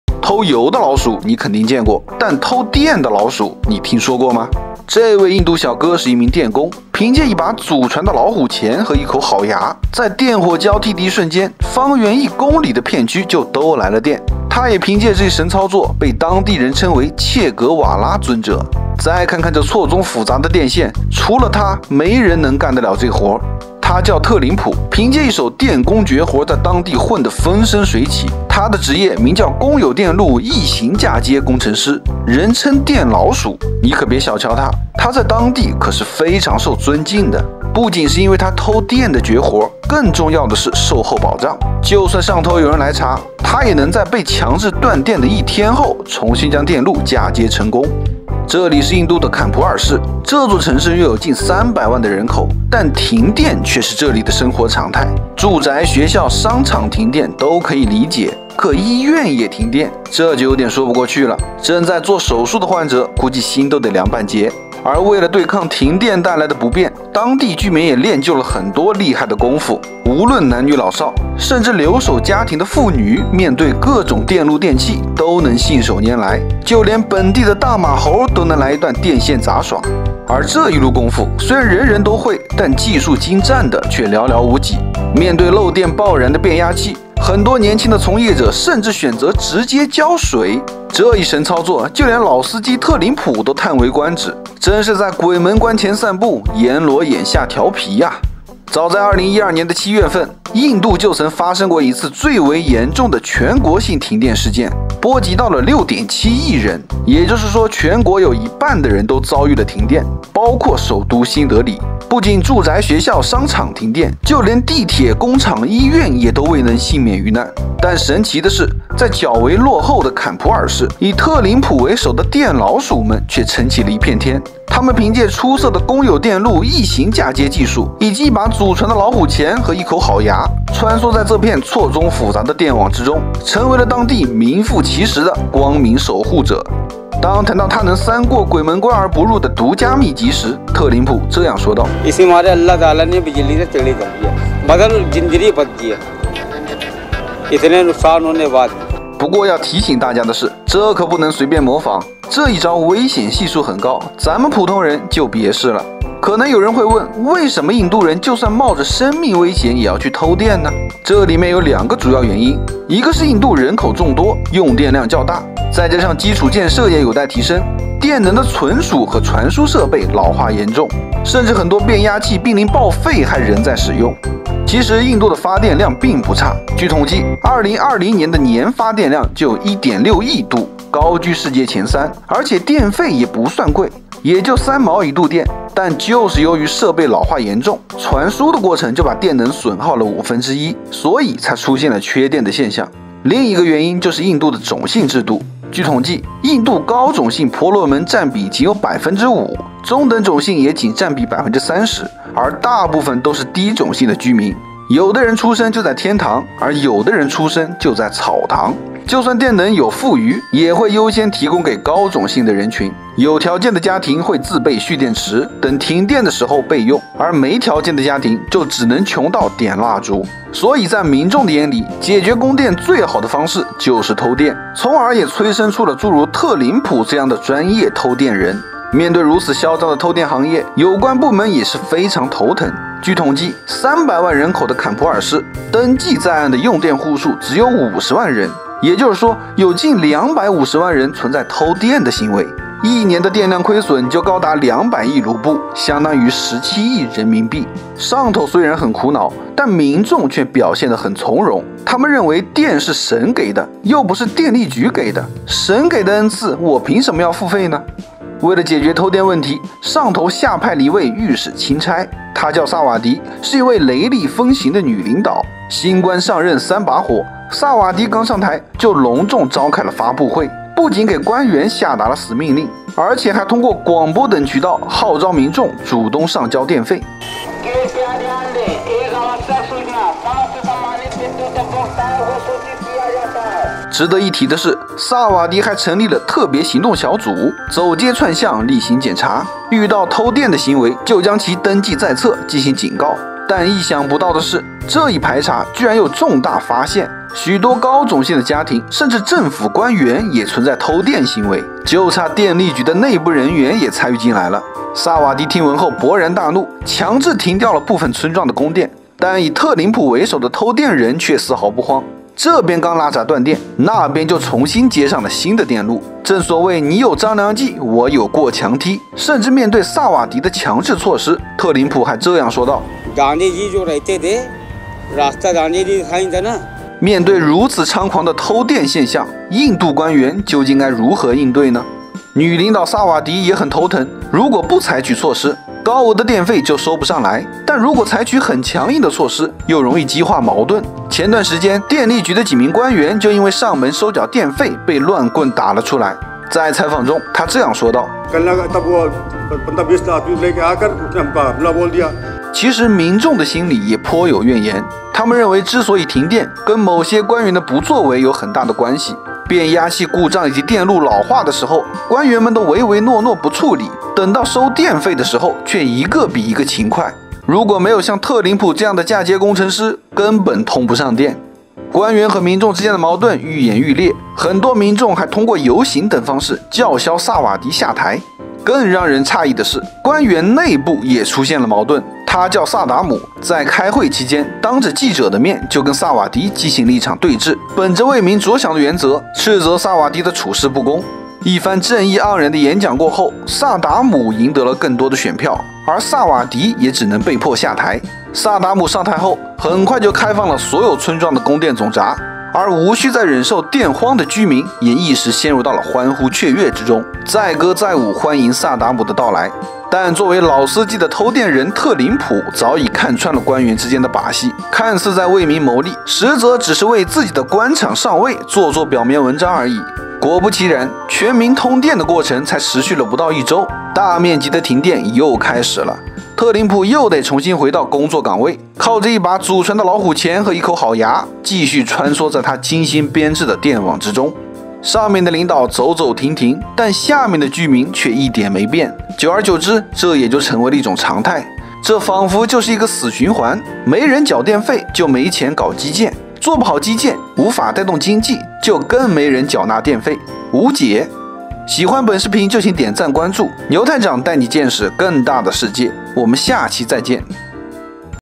印度偷電現象有多瘋狂，專家來了都直呼內行，這操作開掛了
印度缺電太過嚴重，數以萬計的家庭靠偷電為生，這也衍生出了一個新的行業，偷電人。